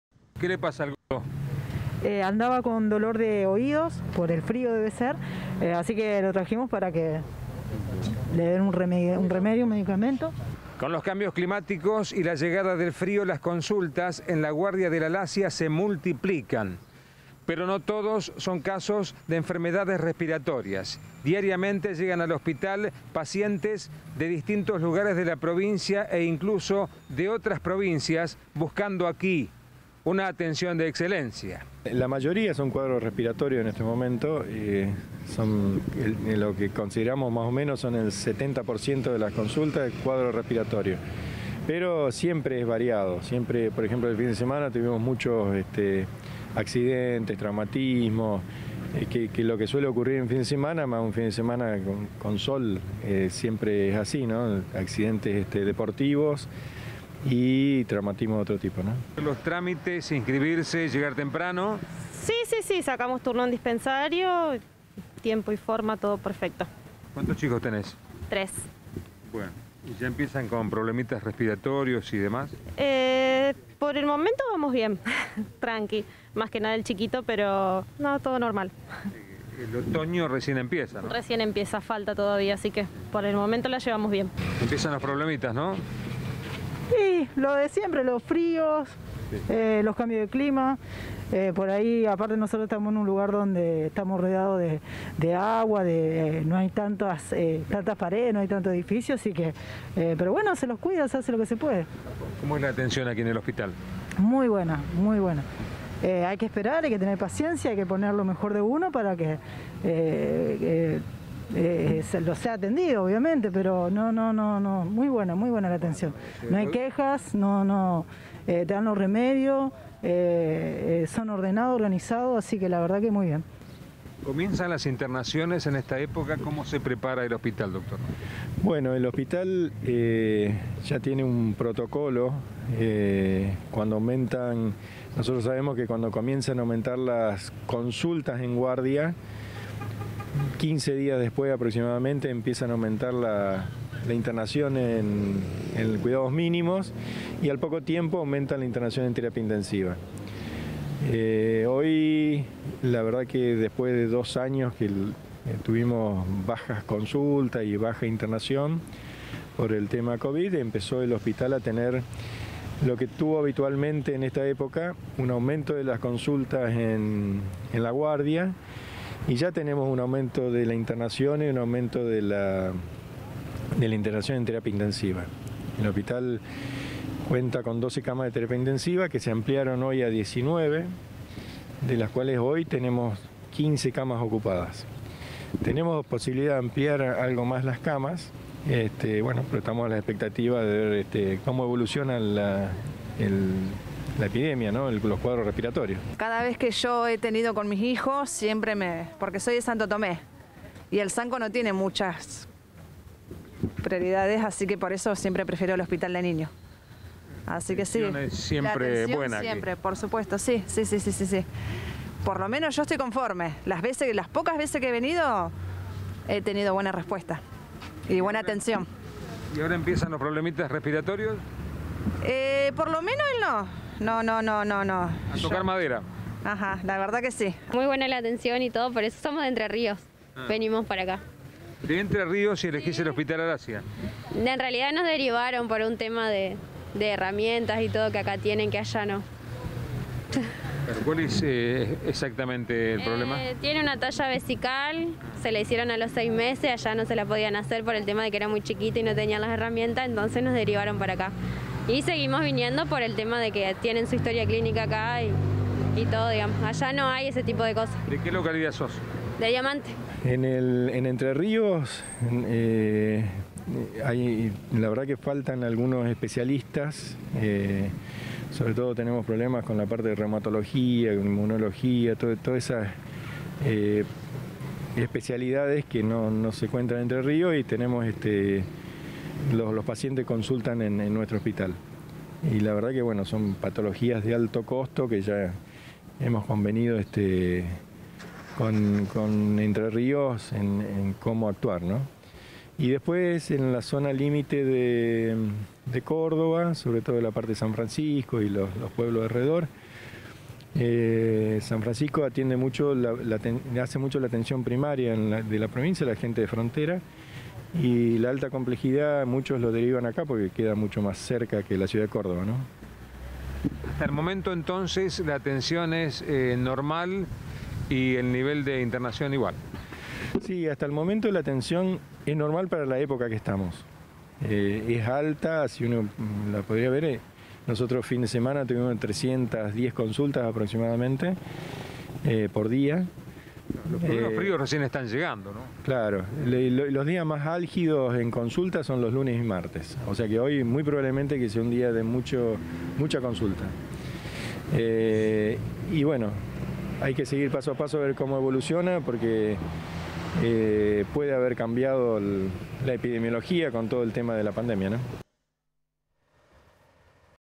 Hospital Alassia: el 70% de las consultas corresponde a cuadros respiratorios
Pese al crecimiento en el número de consultas, como también de las internaciones en cuidados mínimos y en terapia intensiva, desde el Hospital de Niños consideran que "la atención es alta, pero normal para la época en la que estamos".